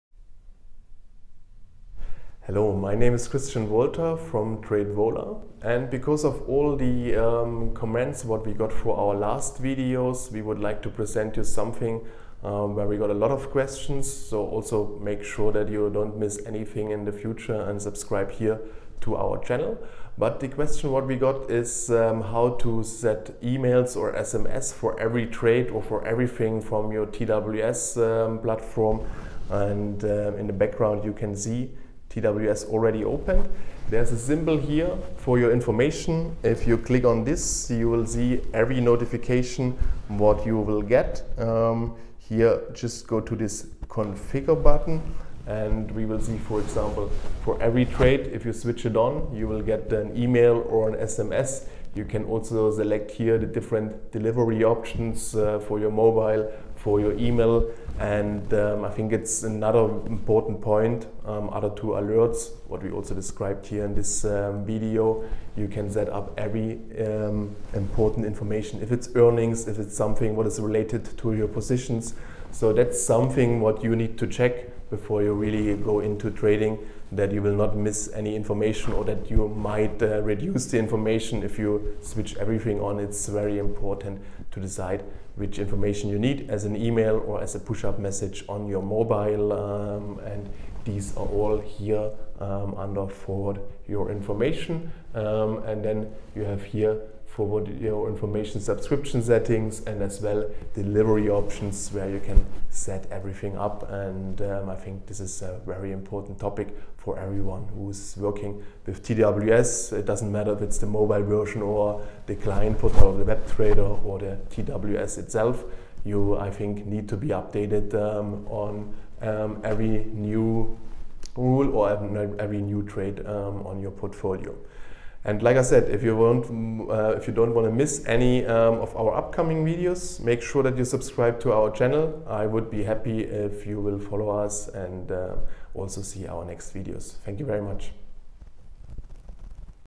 Always stay up to date! | How do I create an alarm function
Learn now how you can keep track of your trades at Interactive Brokers anytime and anywhere and how you can set your notification function.